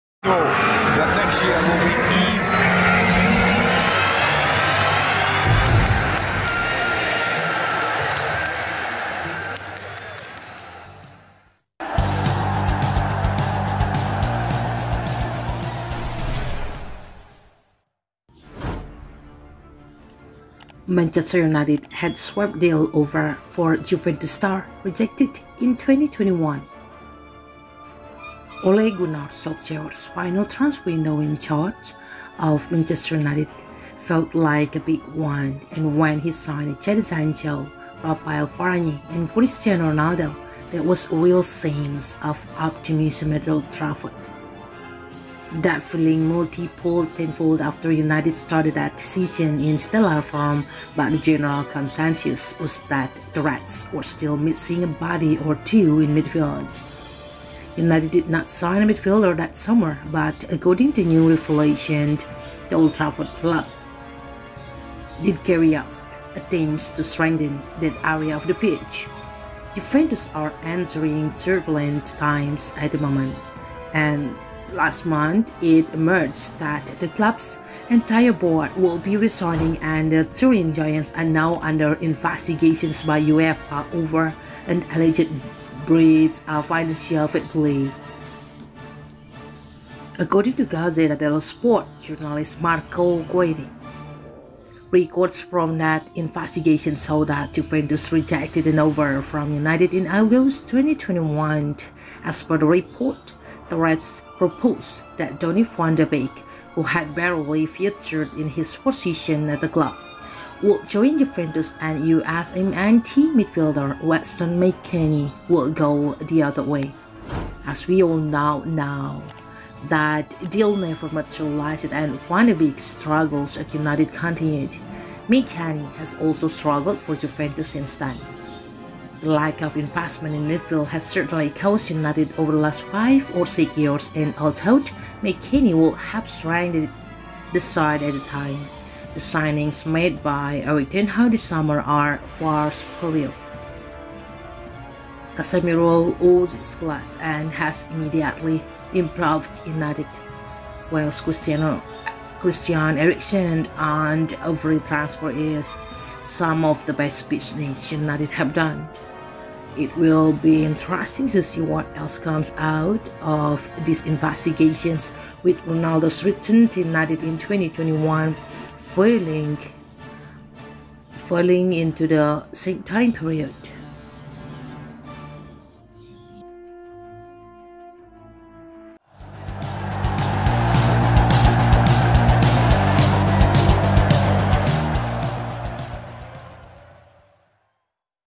Swap Deal: Man United agree swap deal offer for Juventus star in January
Man United had swap deal offer for Juventus star rejected in 2021

Ole Gunnar Solskjaer’s final transfer window in charge of Manchester United felt like a big one and when he signed Jadon Sancho, Raphael Varane and Cristiano Ronaldo, there was a real sense of optimism at Old Trafford.

That feeling multiplied tenfold after United started that season in stellar form but the general consensus was that the Reds were still missing a body or two in midfield.

United did not sign a midfielder that summer but according to new revelations, the Old Trafford club did carry out attempts to strengthen that area of the pitch.

Juventus are enduring turbulent times at the moment and last month, it emerged that the club’s entire board would be resigning and the Turin giants are now under investigation by UEFA over an alleged breach of financial fair play.

According to Gazzetta dello Sport journalist Marco Guidi, records from that investigation show that Juventus rejected an offer from United in August 2021.

As per the report, the Reds proposed that Donny van de Beek, who had barely featured in his first season at the club, would join Juventus and USMNT midfielder Weston McKennie would go the other way.

As we all now know, that deal never materialised and Van de Beek’s struggles at United continued. McKennie has also struggled for Juventus since then.

The lack of investment in midfield has certainly cost United over the last five or six years and although McKennie would have strengthened the side at the time, the signings made by Erik ten Hag this summer are far superior.

Casemiro oozes class and has immediately improved United, whilst Christian Eriksen on a free transfer is some of the best business United have done.

It will be interesting to see what else comes out of this investigation, with Ronaldo’s return to United in 2021 falling into the same time period.